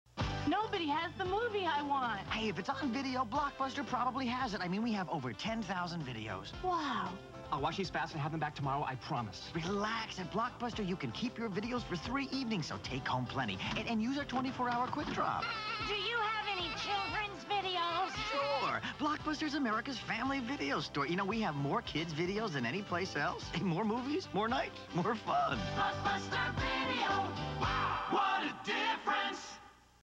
1988 Blockbuster Video Commercial [HQ]
Recorded from an old Betamax Cassette I had in my basement. Old commercials rule!!! I found a new way to record these commercials in high quality 720x480. No more crappy, low resolution videos from me. Enjoy!!!